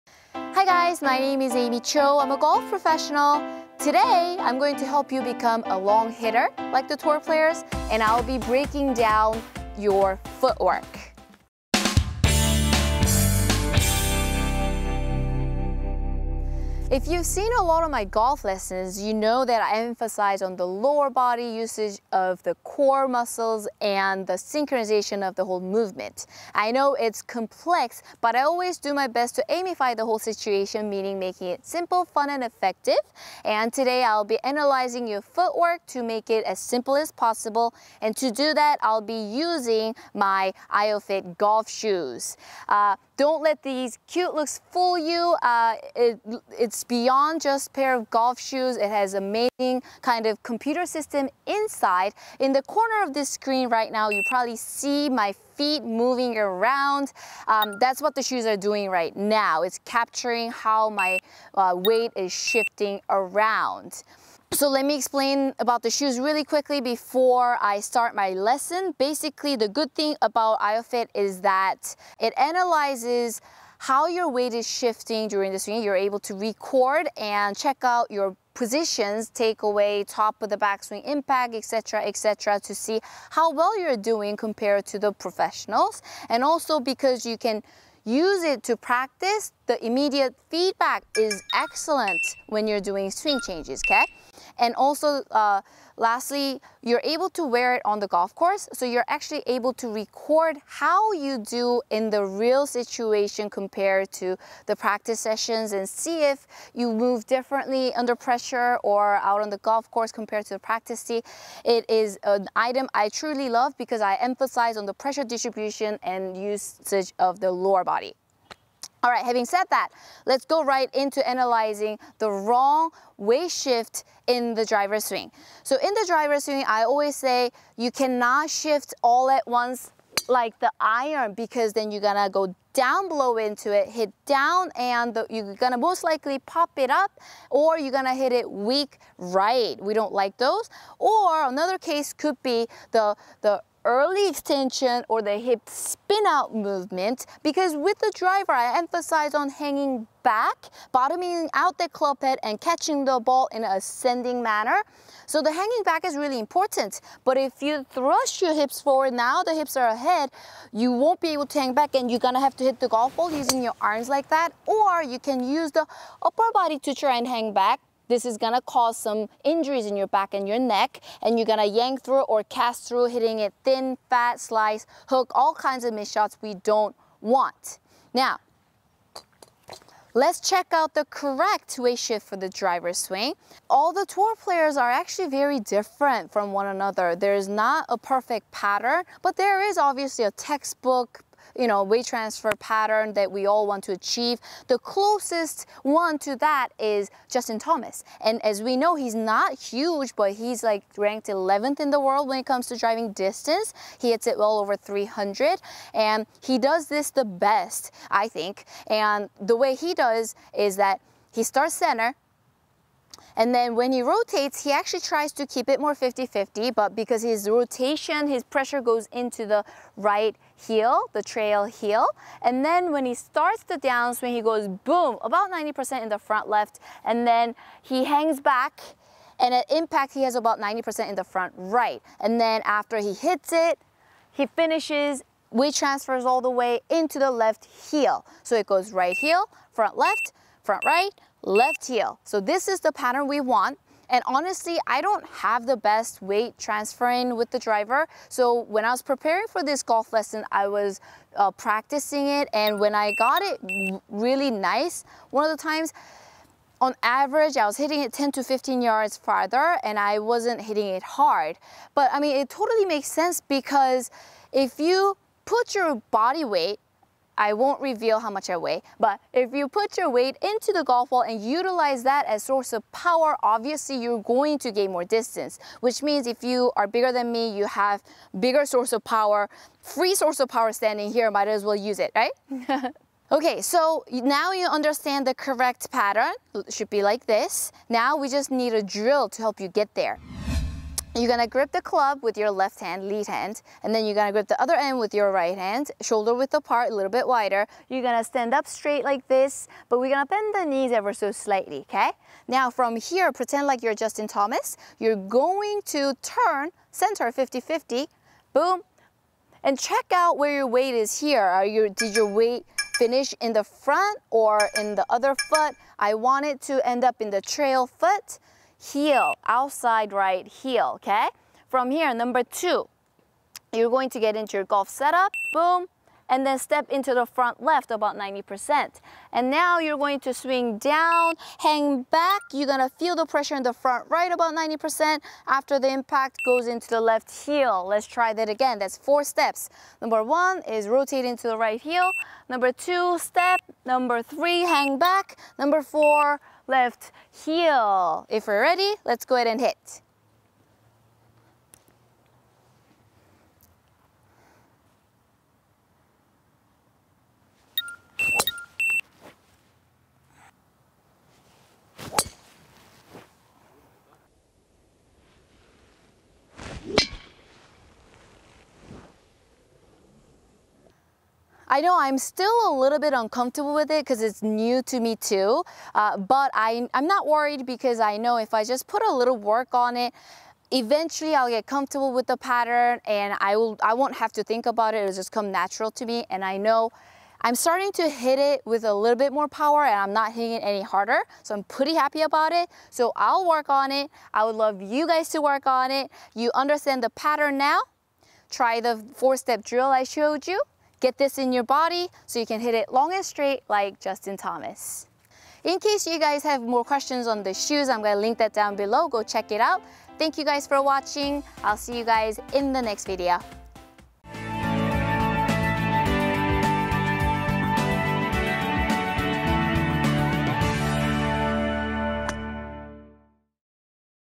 Driver Foot Work & Salted Shoes (IOFIT) | Golf with Aimee
Foot work, balance & weight shift are key to creating effortless power. These are difficult to spot to naked eyes; therefore, these are often overlooked. I will break down why I emphasize back foot to stay down longer with the driver swing. This video will answer a lot of your questions you may have!!

If you would like to check out these Salted Golf Shoes (IOFIT)

Filmed at our beautiful Upland Hills CC
DATA provided by Flightscope